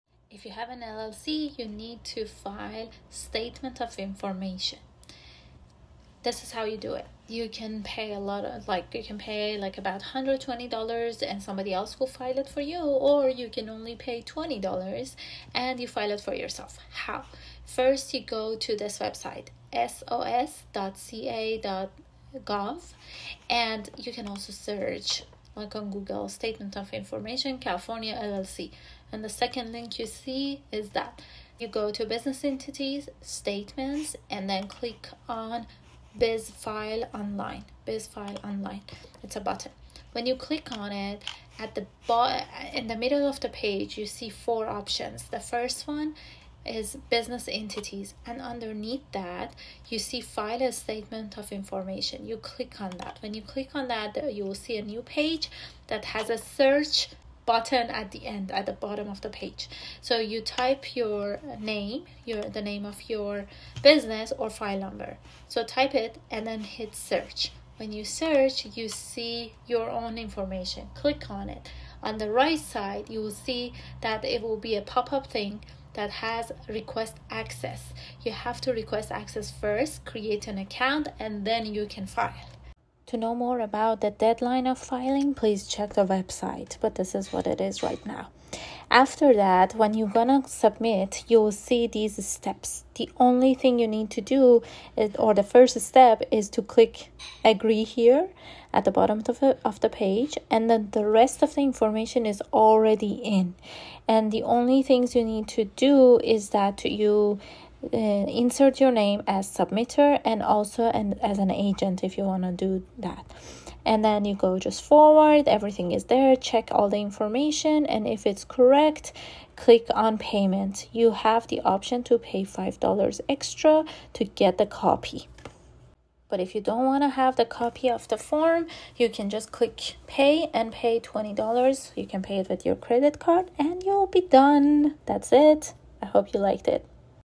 How to file CA Statement of Information yourself in 5 minutes?
How to file CA Statement of Information myself in 5 minutes?